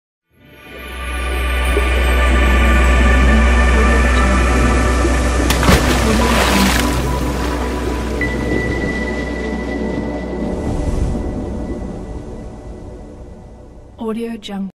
WATER DROP LOGO REVEAL
After Effects CC, CS6 | No Plugin | 1920x1080 | 180 MB
It will be a great inspiration for us to create more useful projects for Customers like you.
The Water Drop Logo Reveal is very easy to edit just replace the logo or any Text and ready to render.It is useful to reveal the logo and text of your company.
Compatible with After Effects, CS6, and Higher
No Plug-ins needed
3D Liquid is prerendered is prerendered
Video tutorial
Fast render time
24 fps